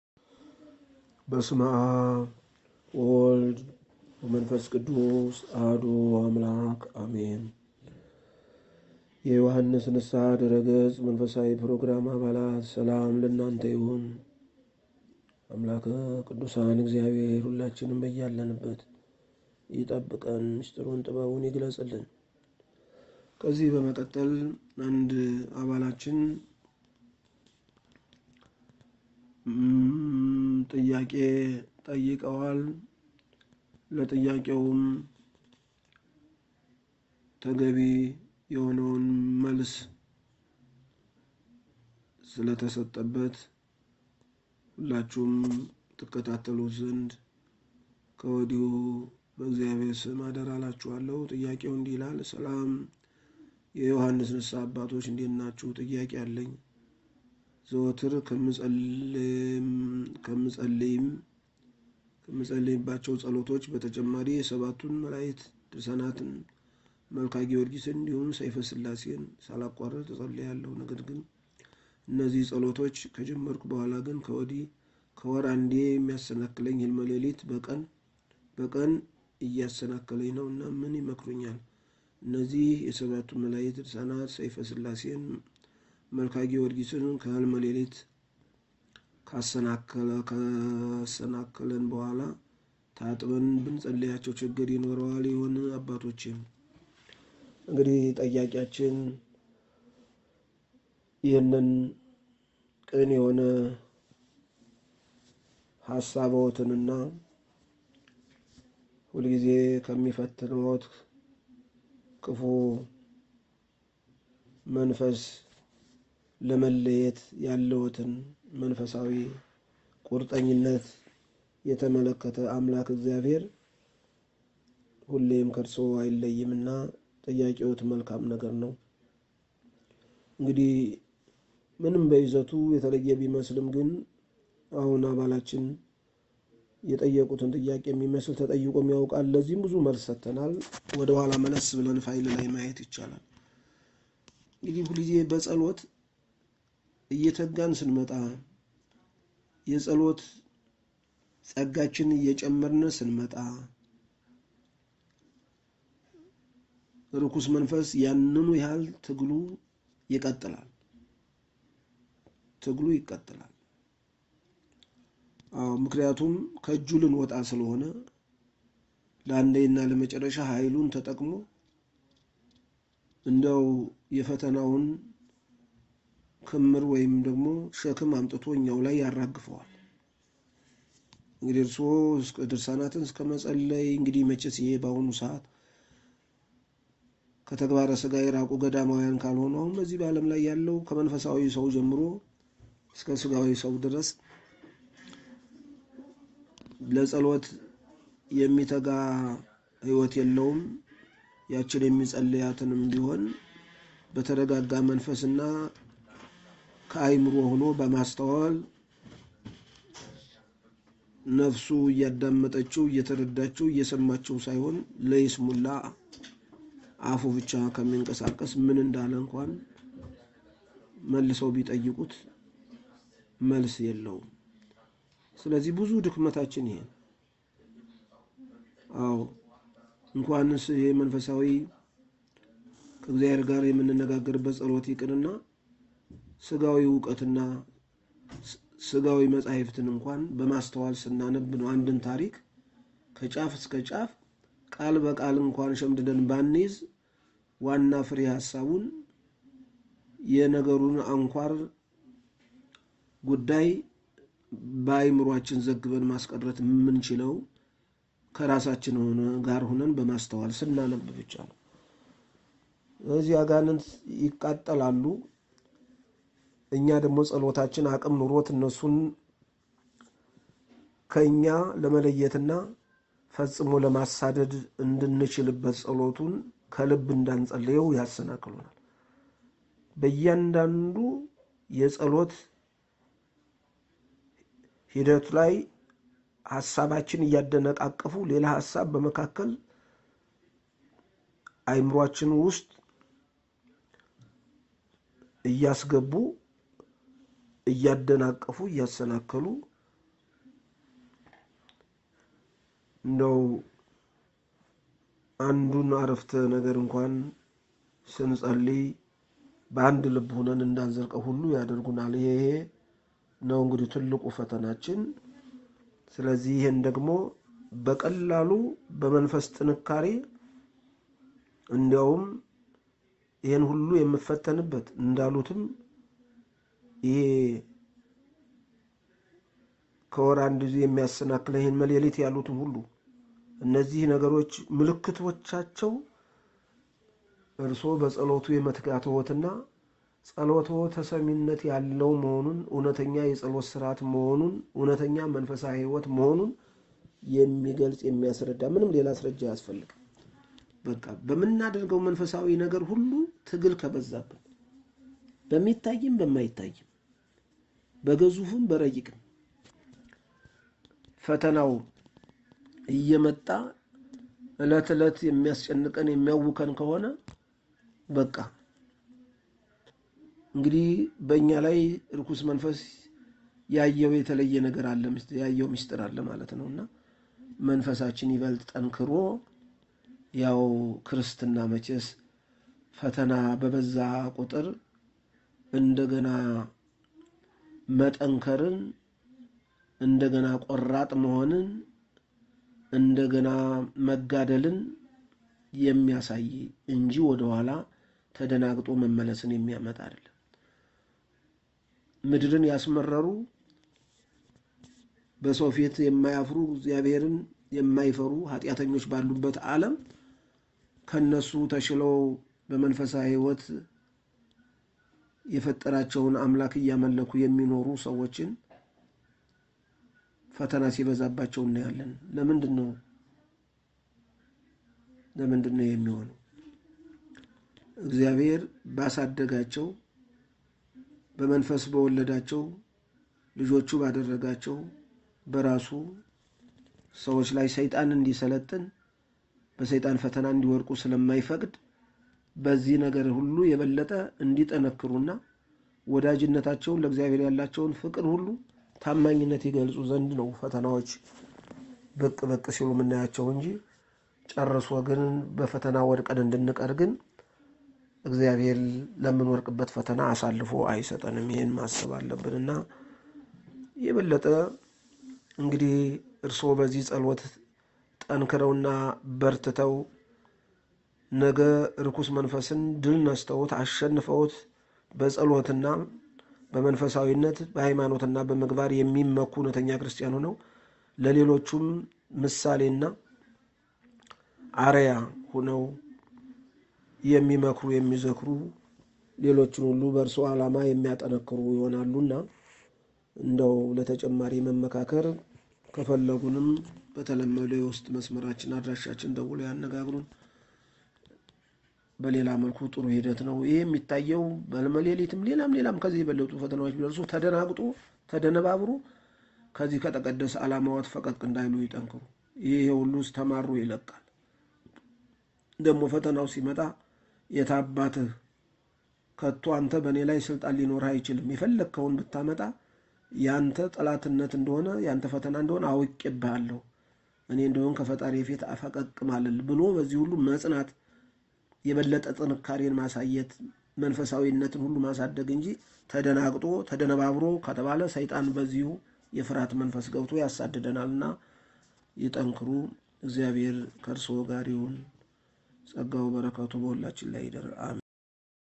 👉🏾ስለ ፀሎቶ"ተጨማሪ ፀሎቶች ከጀመርኩ በኃላ ከወር አንዴ የሚያሰናክለኝ ህልመ ሌሊት በቀን በቀን እያሰናከለኝ ነውና ምን ይመክሩኛል❓”
የዮሐንስ ንስሐ ድረገጸ መንፈሳዊ ትምህርት አና ምክር እንዲሁም ለጥያቄዎ መልስ ለማግኘት ይህን ሊንክ በመጫን ማህበራዊ ገጾቻችንን ይከታተሉ፦